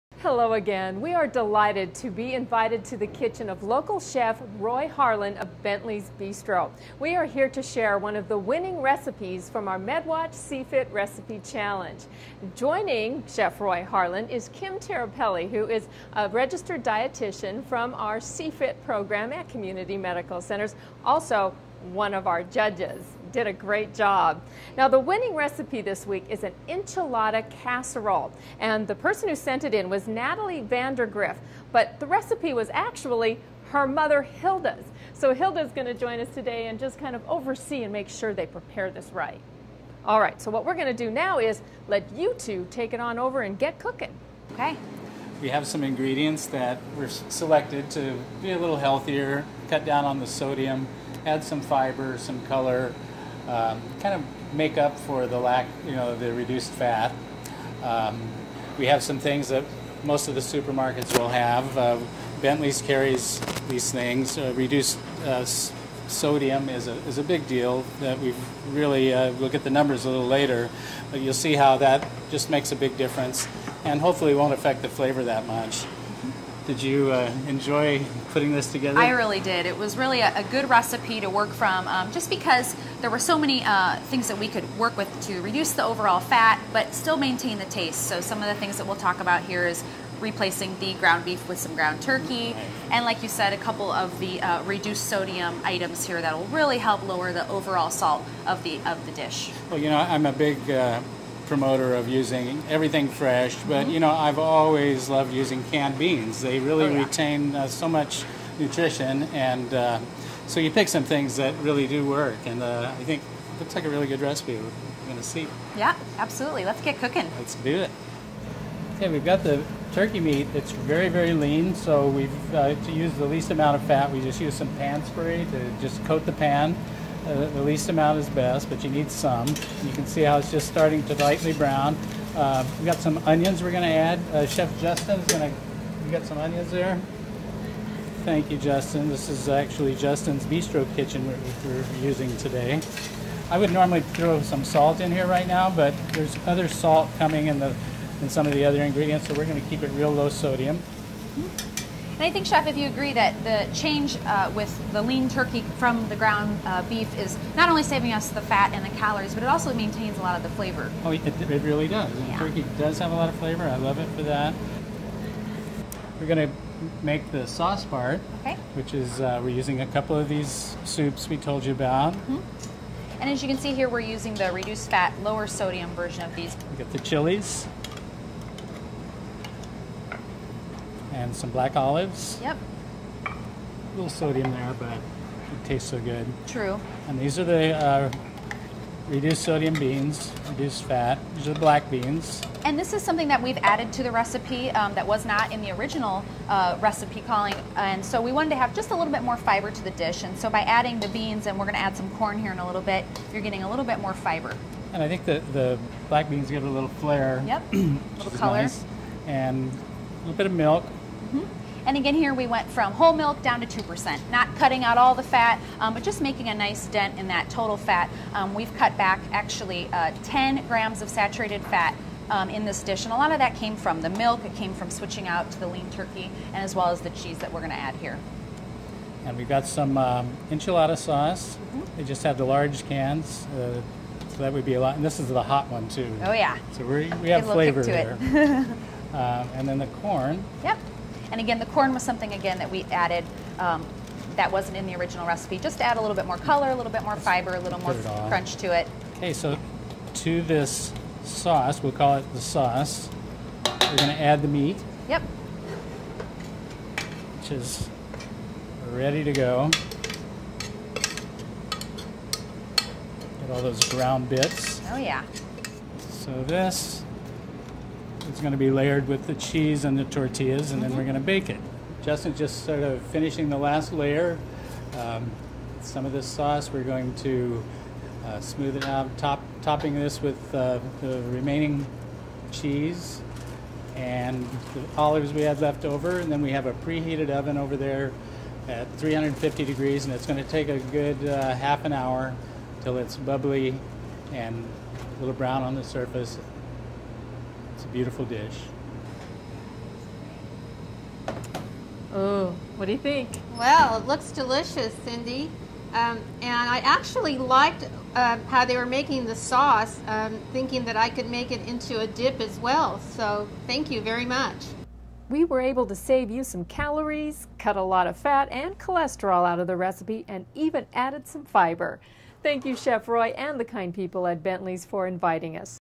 C-Fit Recipe Challenge: Enchilada Casserole
MedWatch is in the kitchen as we prepare the winning entries from our C-Fit Recipe Challenge. Proof that you can enjoy old family favorites without the guilt.